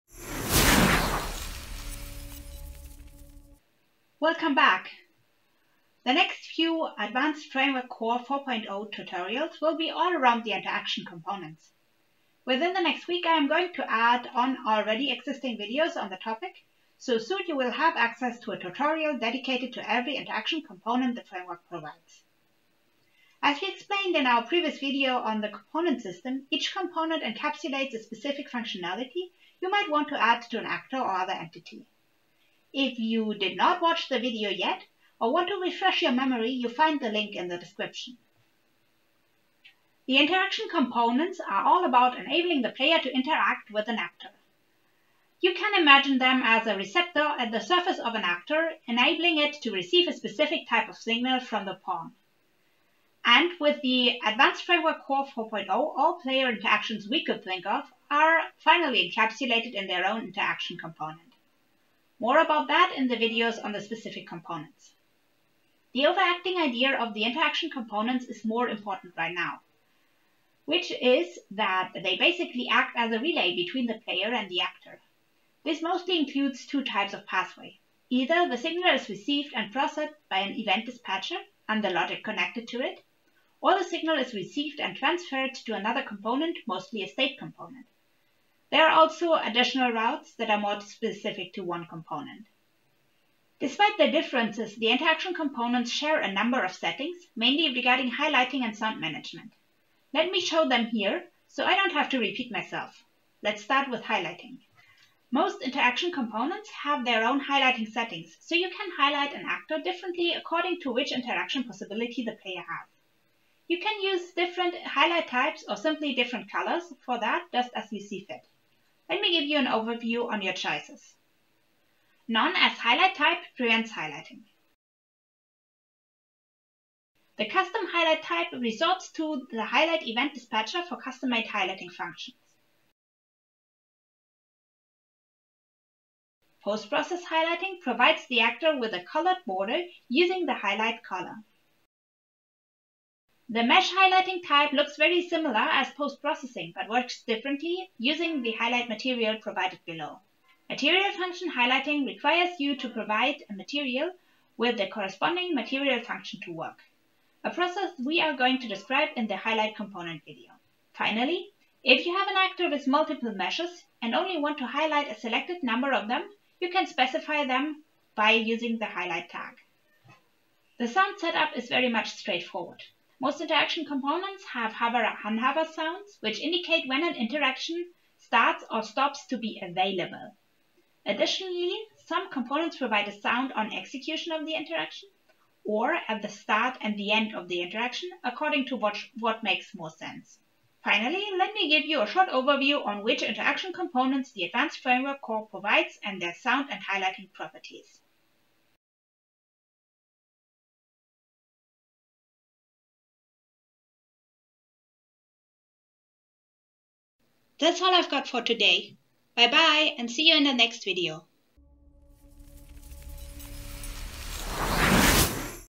Interaction Components (Advanced Framework Core Tutorial)
A short introduction to the concept and a few shared properties of interaction components in the Advanced Framework Core.

- - - This tutorial applies to the Advanced Framework Core 4.0, 4.1 and 5.0. - - -

If you have any questions or need help, join our lovely Discord community: